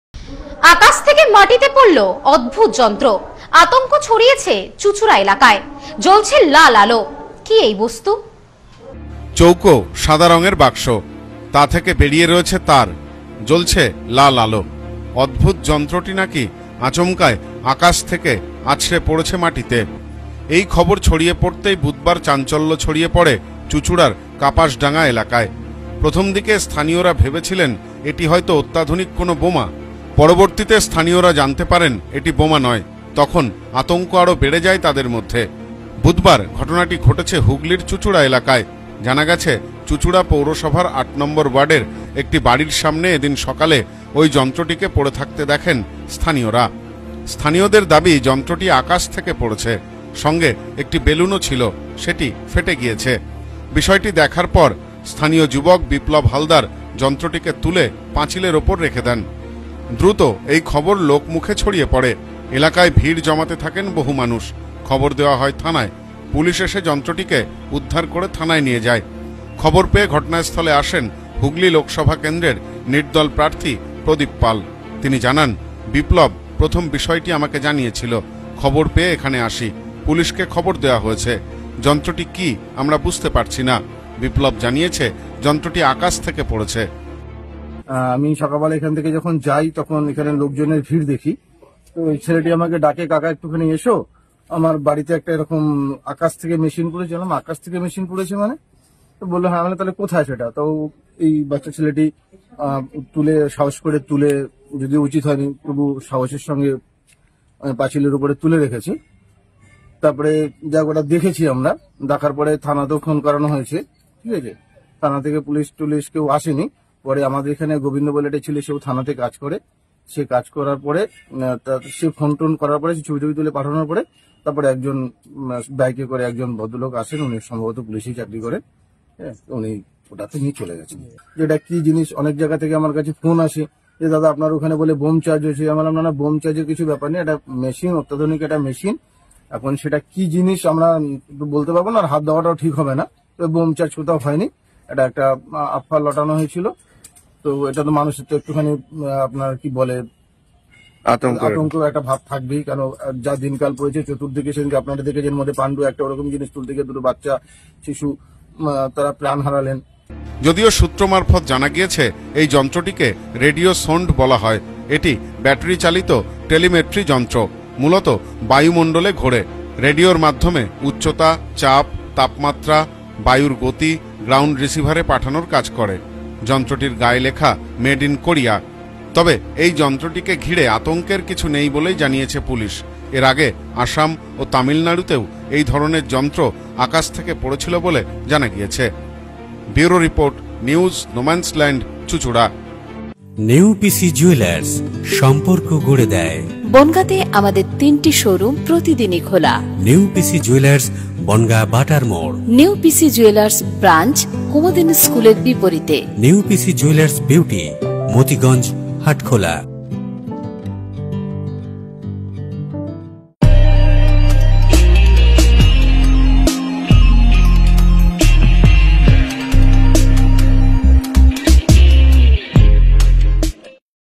আকাশ থেকে মাটিতে পড়লো অদ্ভুত যন্ত্র!
আকাশ থেকে মাটিতে পড়লো অদ্ভুত যন্ত্র! আতঙ্ক চুঁচুড়া এলাকায়। জ্বলছে লাল আলো। কি এই বস্তু?